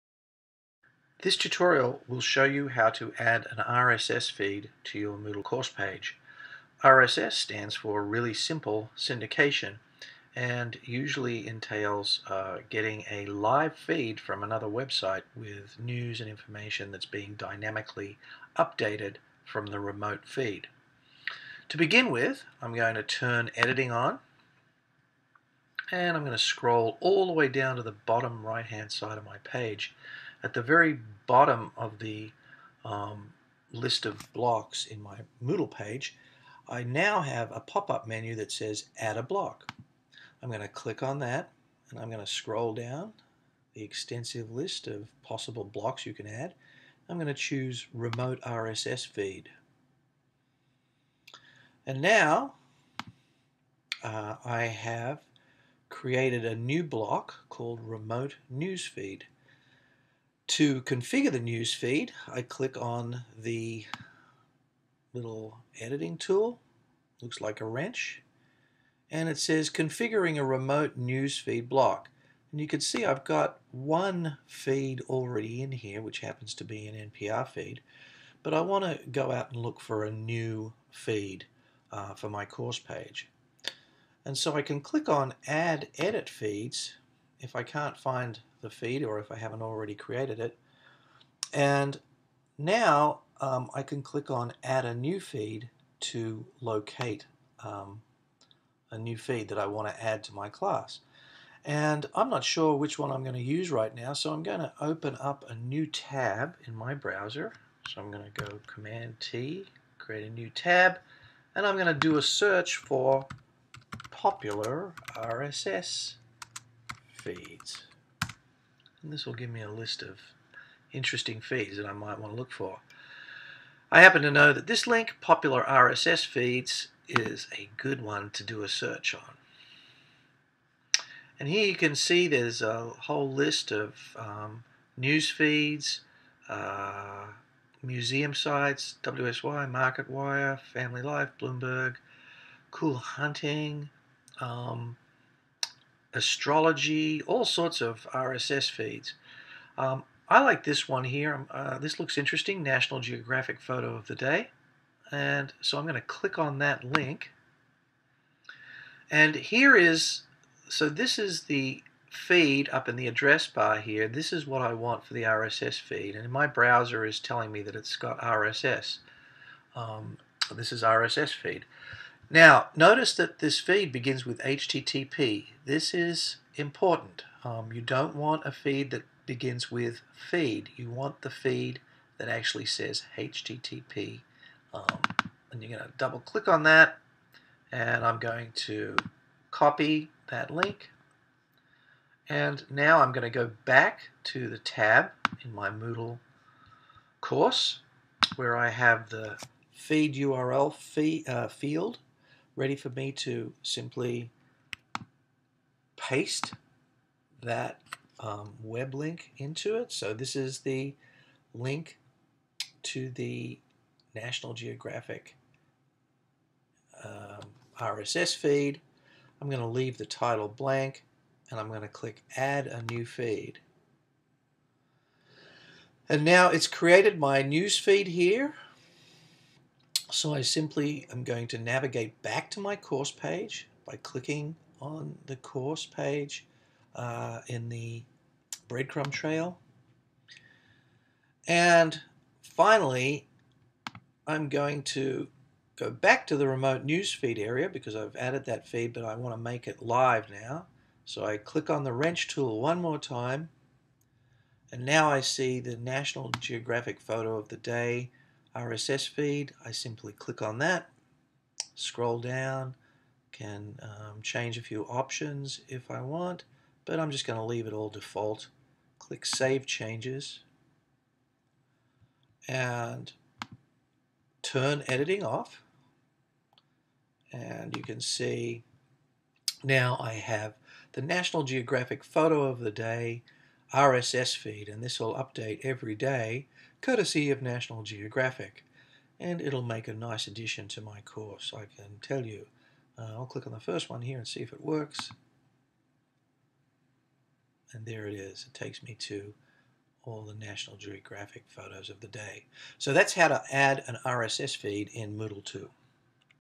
Adding an RSS Feed in Moodle 2
How to add an RSS feed to your Moodle 2 course page